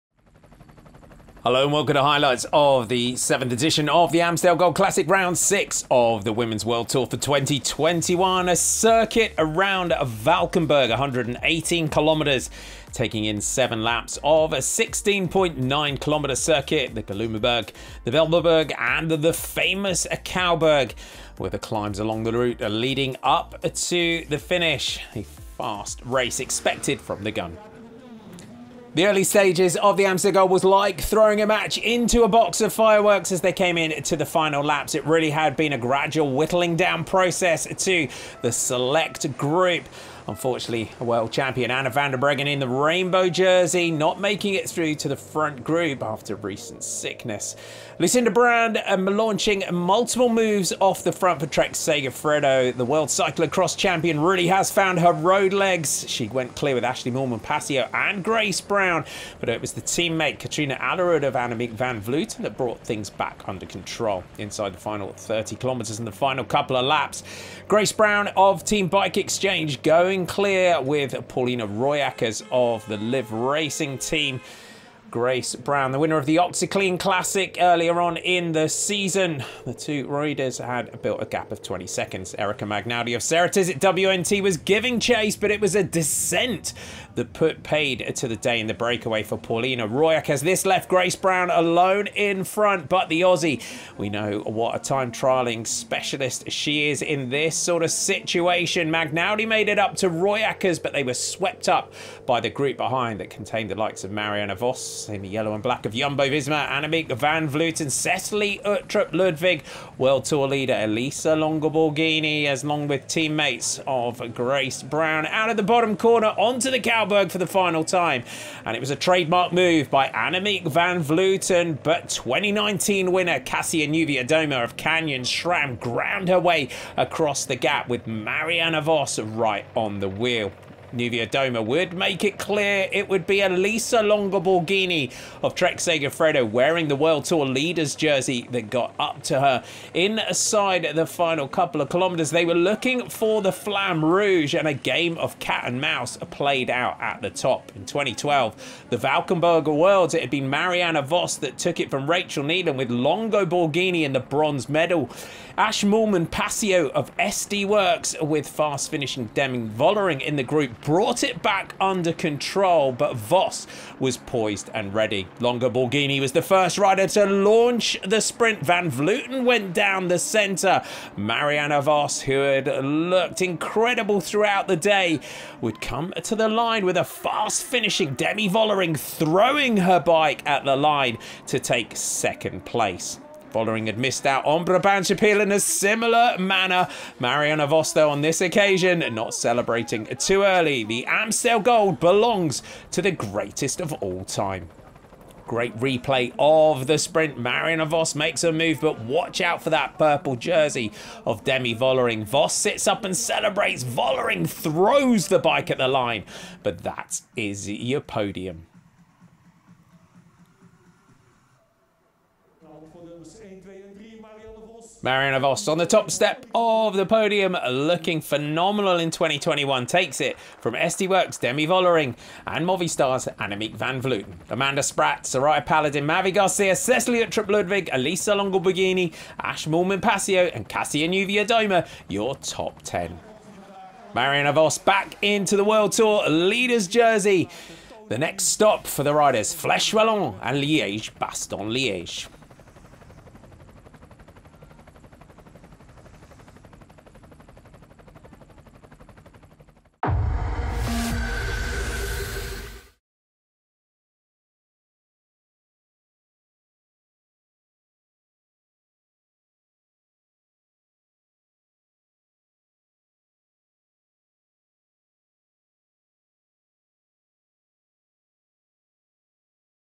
Amstel Gold Race 2021 | Elite Women | Highlights | Cycling | Eurosport
Highlights from the elite women's race of the 2021 Amstel Gold Race.